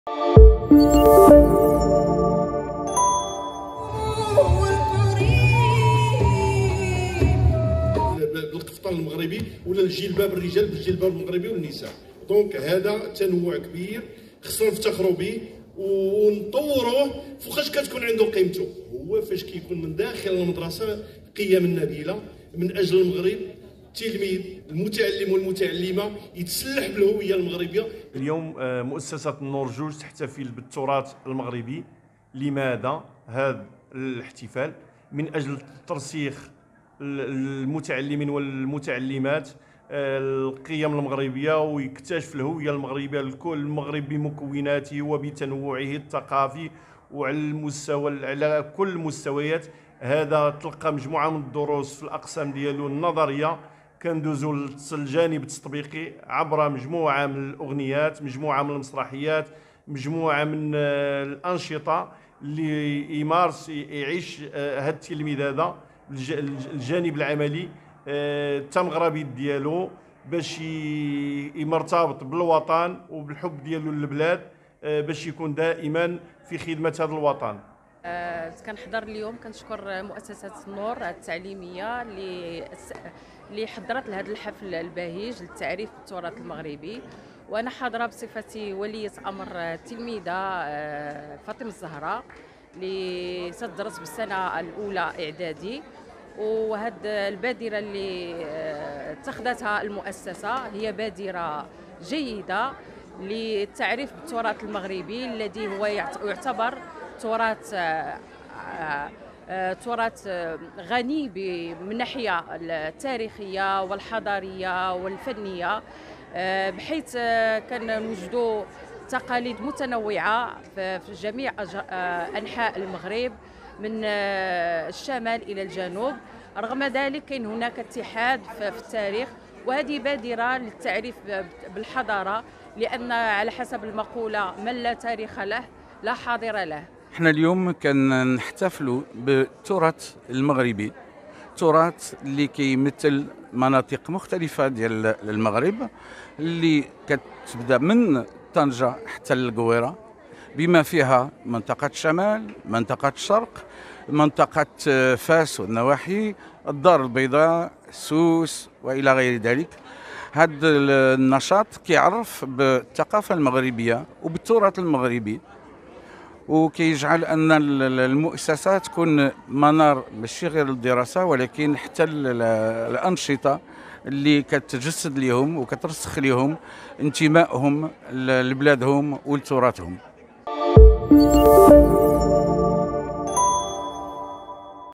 البيضاء..تلاميذ مؤسسة النور الخاصة يحتفلون بالثرات المغربي العريق
نظمت مؤسسة النور الخاصة حفلا للتعريف  بالتراث المغربي العريق، وضم الحفل مجموعة من الفقرات عرفت بالهوية المغربية.